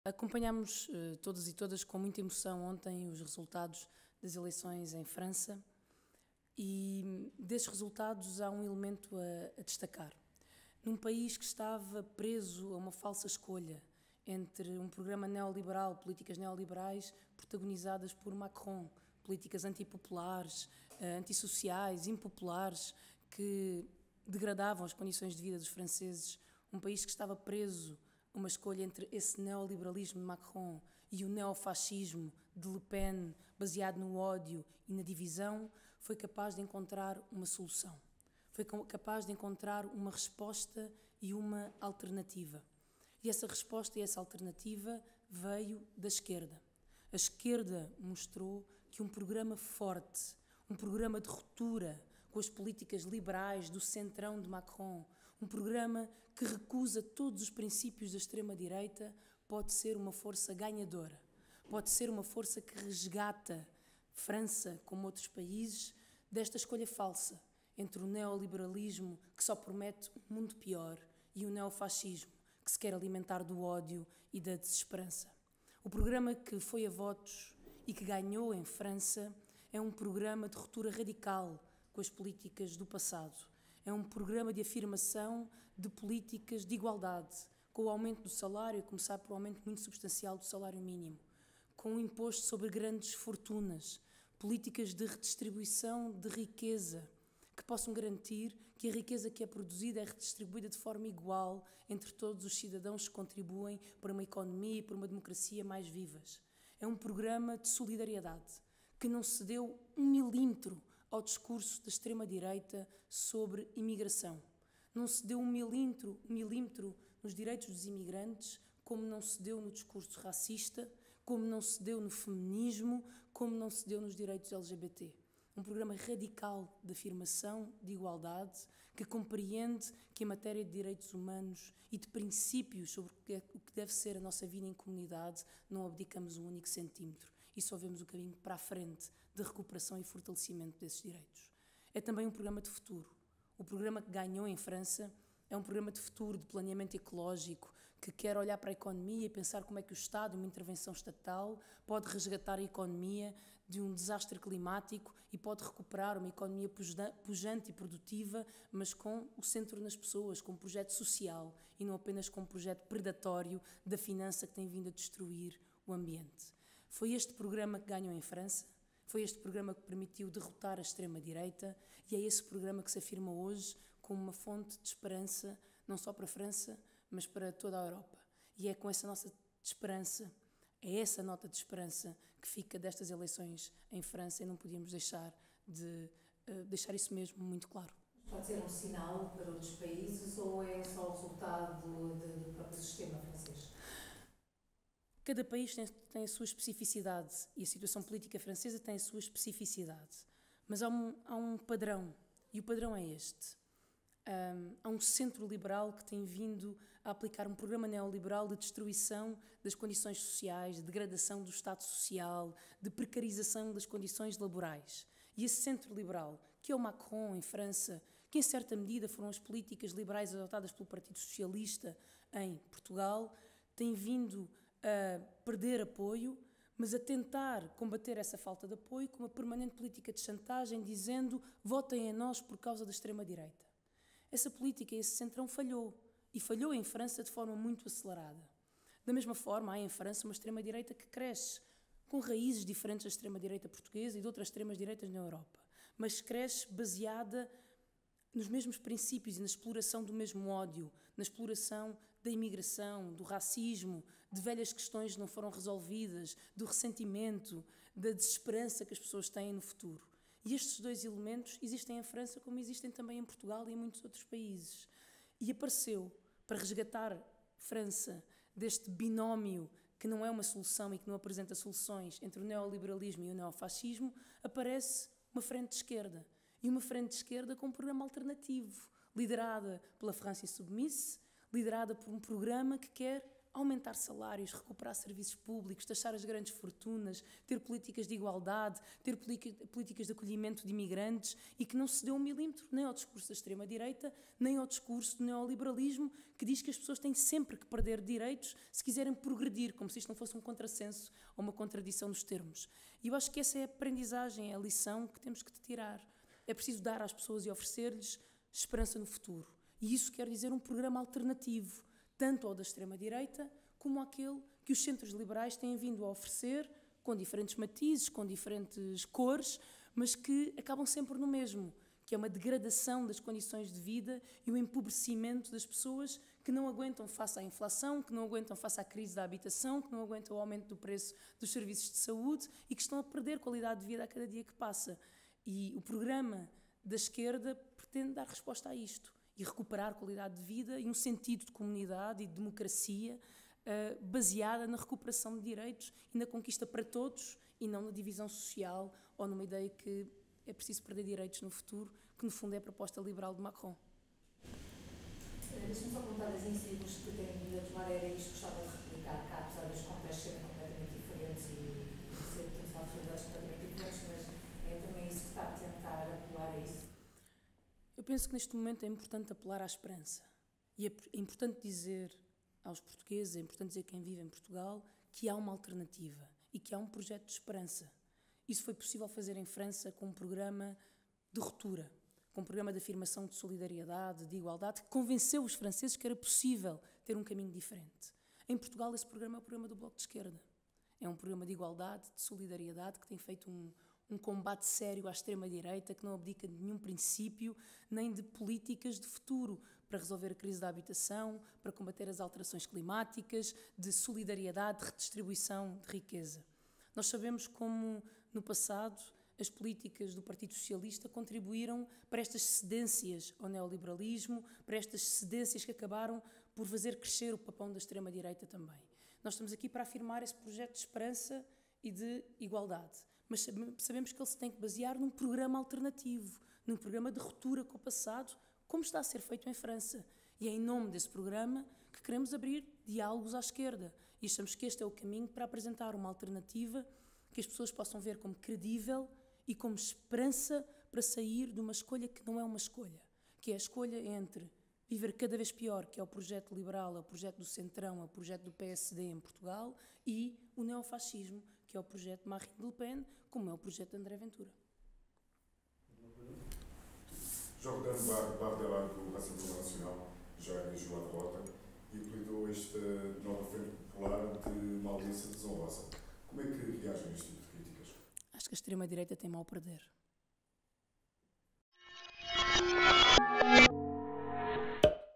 Entre o neoliberalismo e neofascismo, os franceses encontraram a esperança na esquerda
Uma esquerda que apresentou um programa forte, um programa de rutura com as políticas liberais do centrão de Macron, que recusa todos os princípios da extrema-direita, pode ser uma força ganhadora.

Uma fonte de esperança, não só para a França, mas para toda a Europa.